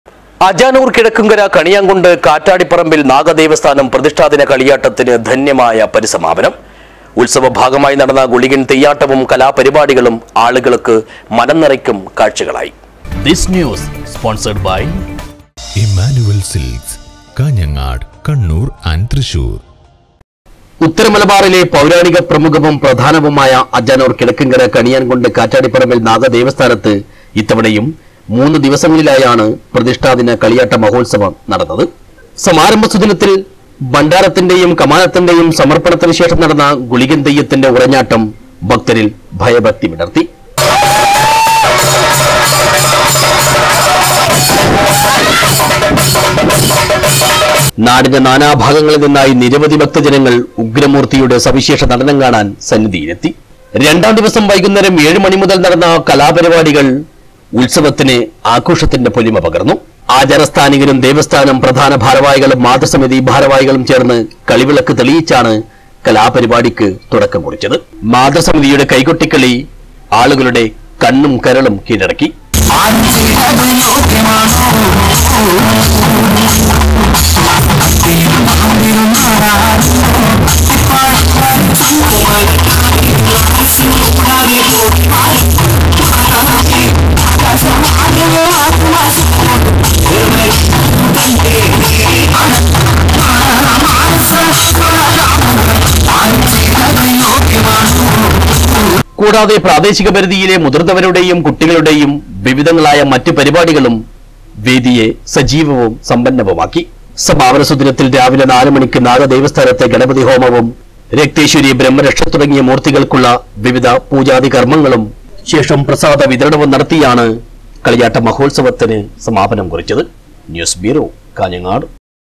അജാനൂര്‍ കിഴക്കുംകര കണിയാംകുണ്ട് കാറ്റാടിപ്പറമ്പില്‍ നാഗദേവസ്ഥാനം പ്രതിഷ്ഠാദിന കളിയാട്ടത്തിന് സമാപനം
അജാനൂര്‍ കിഴക്കുംകര കണിയാംകുണ്ട് കാറ്റാടിപ്പറമ്പില്‍ നാഗദേവസ്ഥാനം പ്രതിഷ്ഠാദിന കളിയാട്ടത്തിന് ധന്യമായ പരിസമാപനം. ഉത്സവ ഭാഗമായി നടന്ന ഗുളികന്‍ തെയ്യാട്ടവും കലാപരിപാടികളും ആളുകള്‍ക്ക് മനം നിറയ്ക്കും കാഴ്ച്ചകളായി.